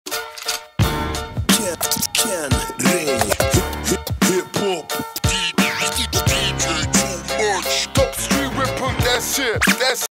Ken Ring ft. DJ 2Much - Sean John x Top Streetwear Commercial MTV Sweden
U.S. fashion giant Sean John presents, in collaboration with its official European retailer Top Streetwear, a new campaign launched on MTV in Sweden and Finland between October to December 2009 and is expected to reach about 7 million viewers.